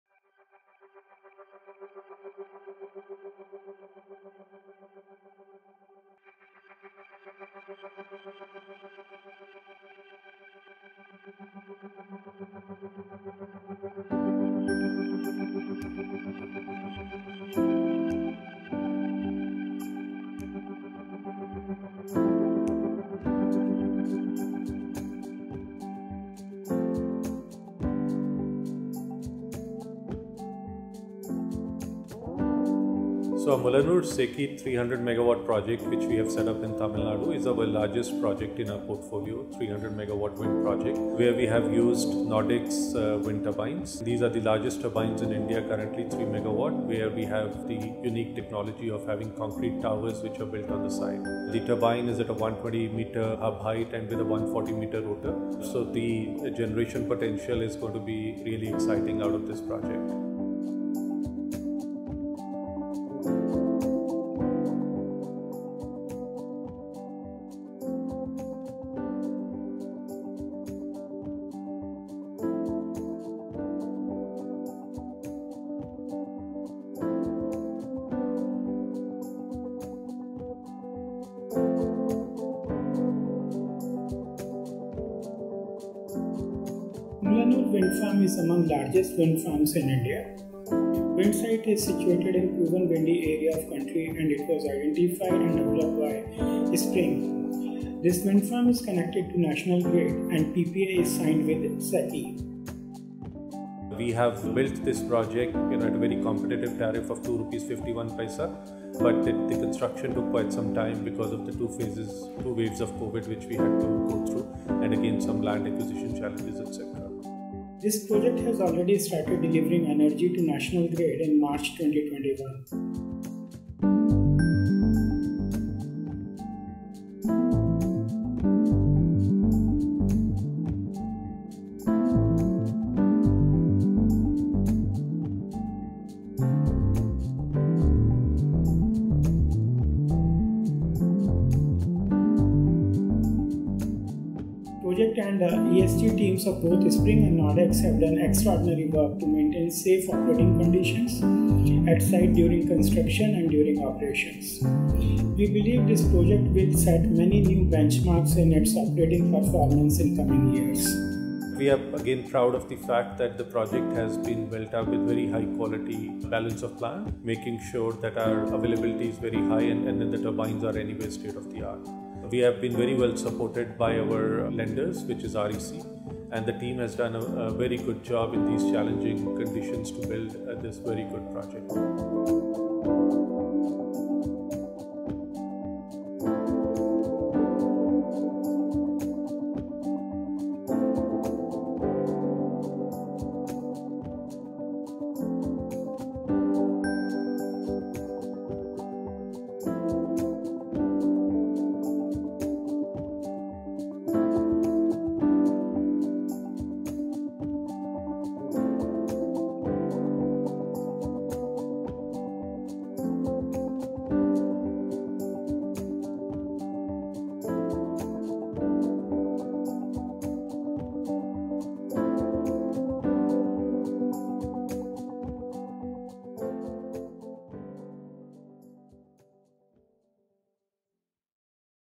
300 MW Sprng Renewable Energy Private Limited
This is a 300 MW wind power project in Tamilnadu with unique concrete technology and houses India's largest commissioned rotor diameter currently of 140 m. The project comprises of 100 nos. of AWP -140 3 MW turbines and is spread across an area of 25 kms.